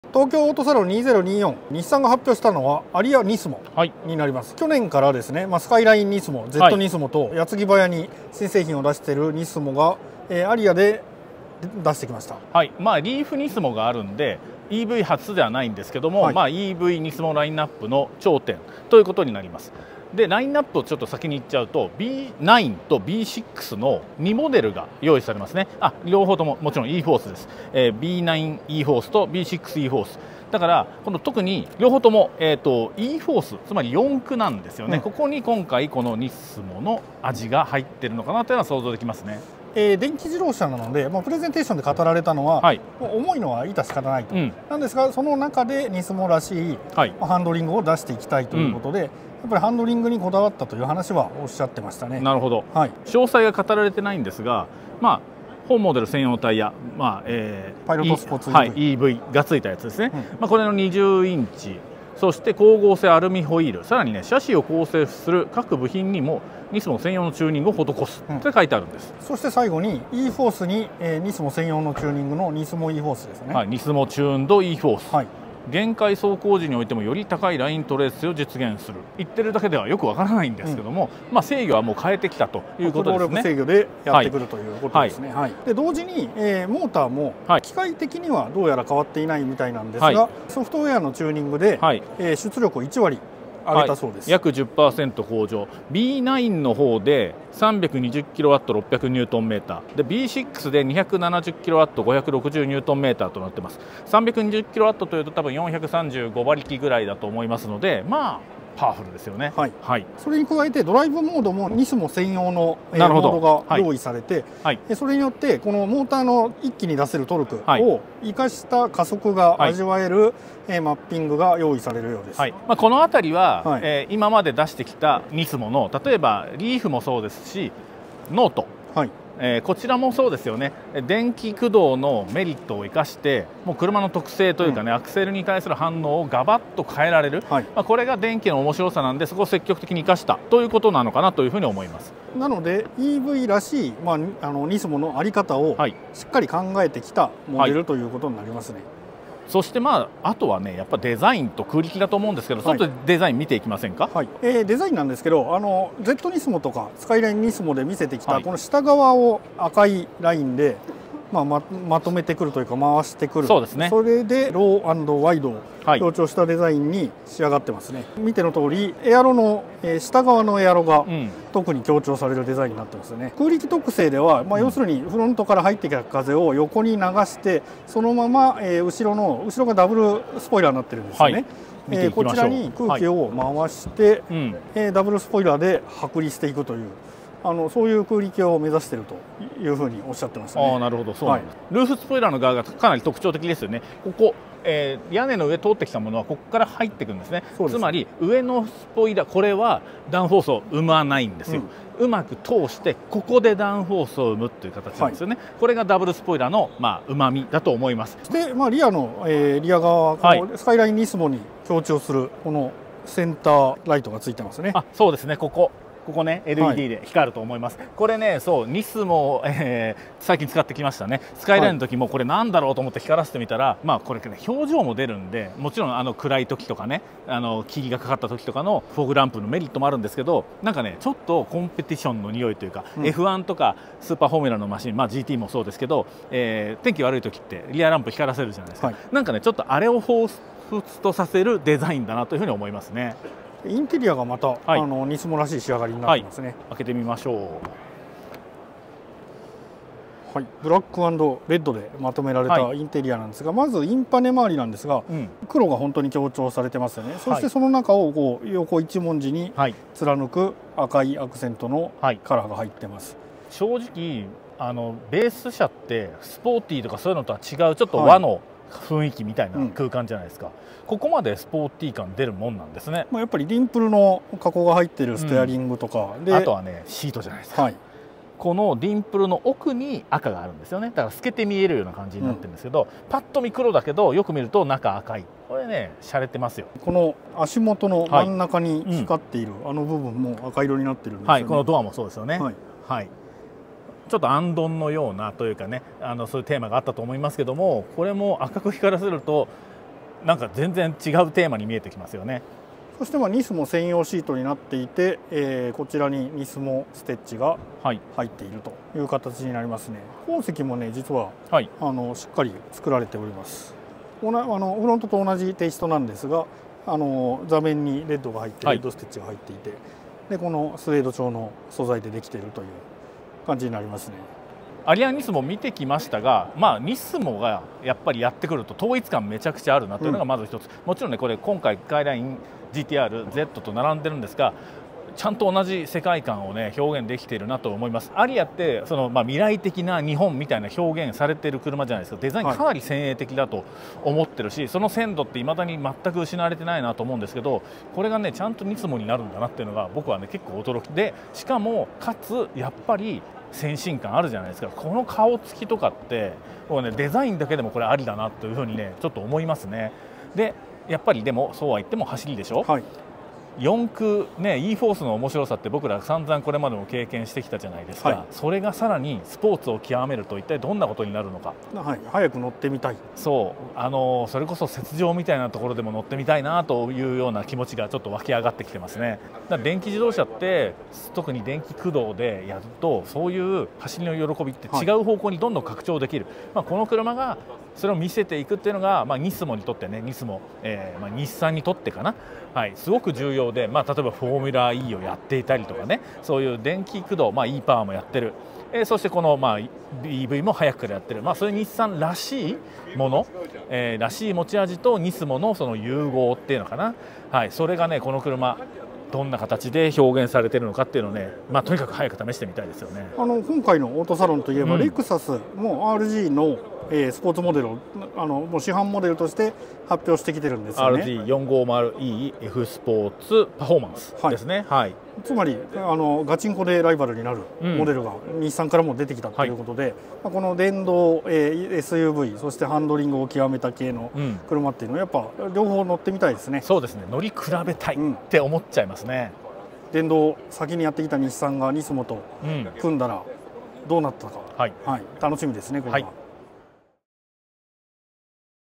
日産 アリア NISMO 詳細解説 by 島下泰久 × 難波賢二
東京オートサロン2024でデビューした日産 アリア NISMO。リーフNISMOに続くBEVモデルは、洗練された空力と、NISMOが独自にセットアップした駆動力制御システムe-4ORCEによって、一体どんな走りを実現しているのでしょうか？